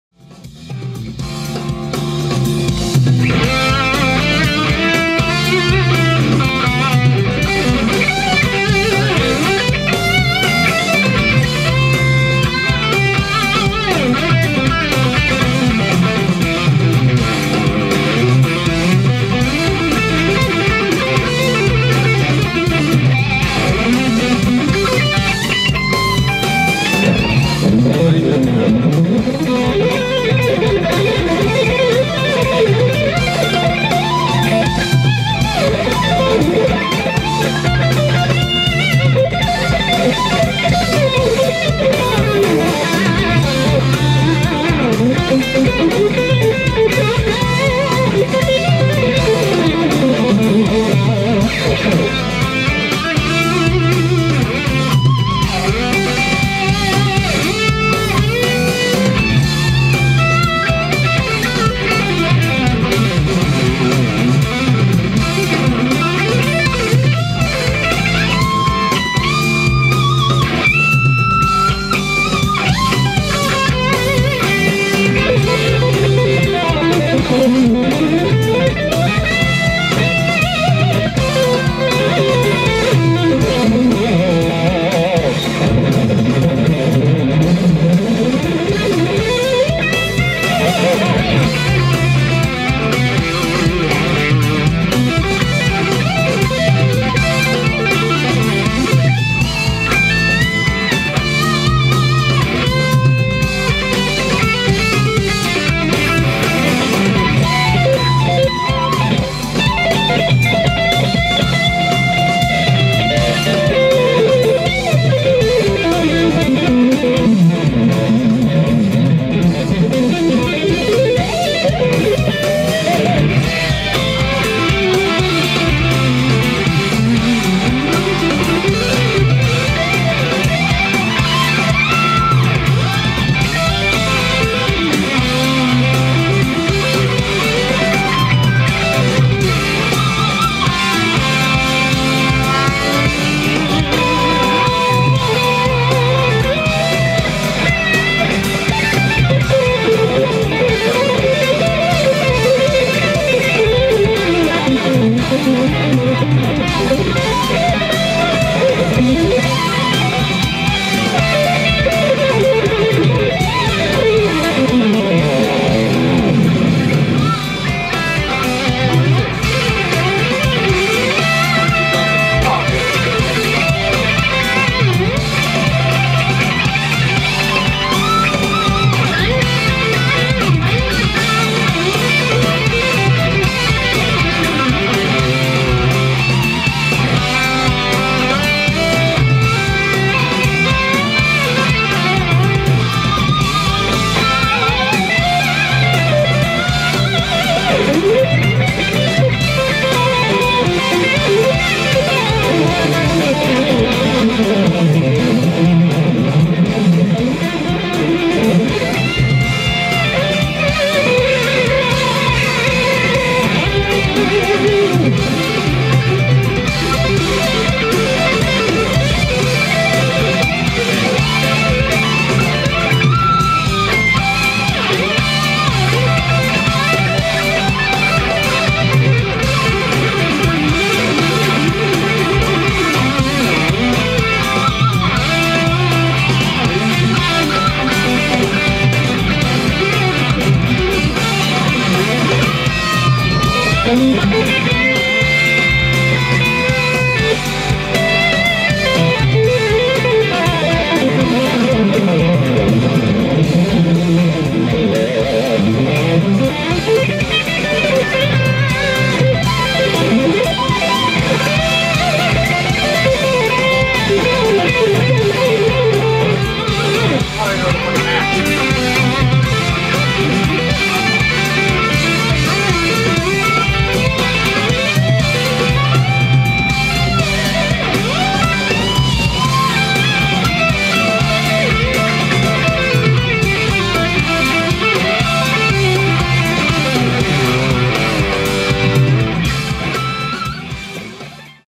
James Ryan and John Sanders Jam 2nd May
Finally Johnny Sanders and I found time for some more jamming!
John is going through a Fender Mustang 4 and I am using the Axe Fx II